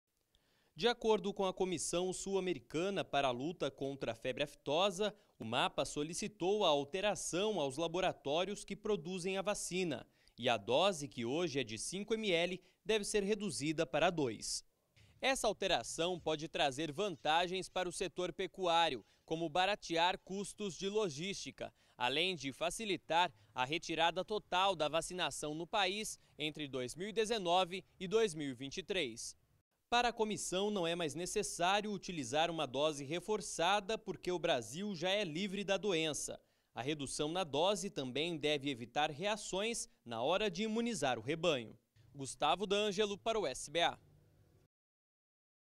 AFTOSA REDUÇÃO DOSE
A dose da vacina contra febre aftosa vai sofrer redução.